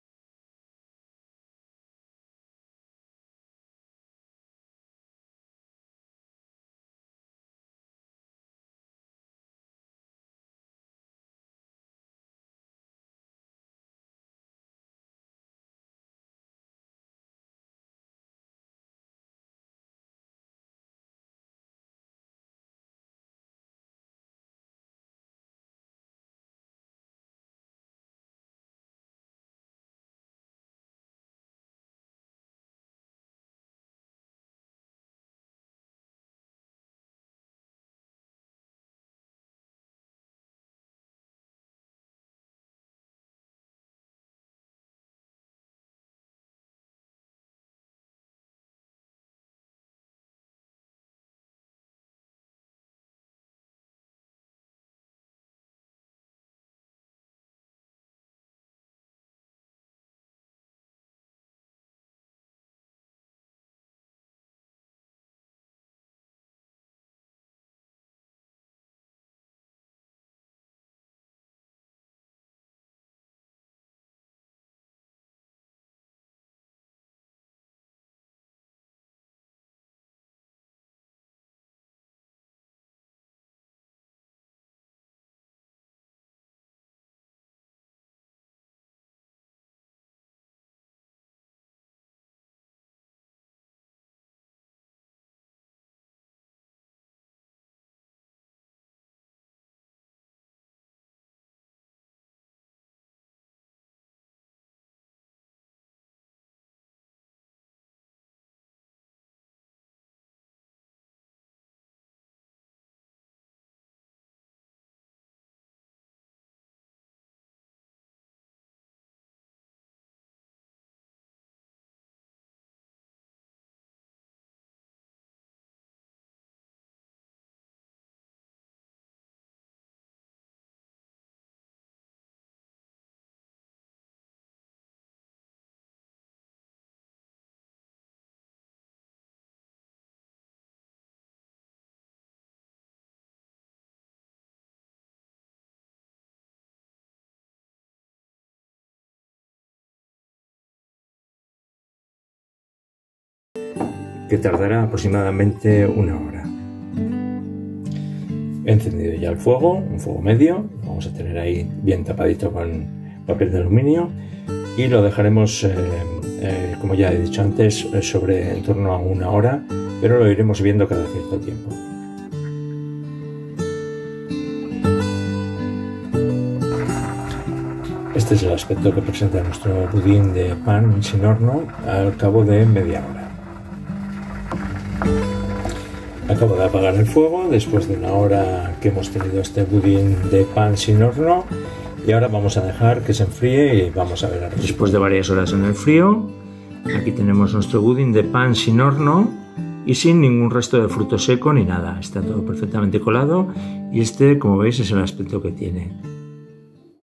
#14. EL MEJOR BUDIN DE PAN SE HACE SIN HORNO.
Ingredientes

400 ml de leche
75 gr de pan duro
2 huevos
Extracto de vainilla
Azúcar para el caramelo
Azúcar para la mezcla. 75 grs
60 g de frutos secos
20 ml de brandy

Seria conveniente añadir los huevos después de colar la mezcla.
Dejar enfriar en frigorífico al menos 4 horas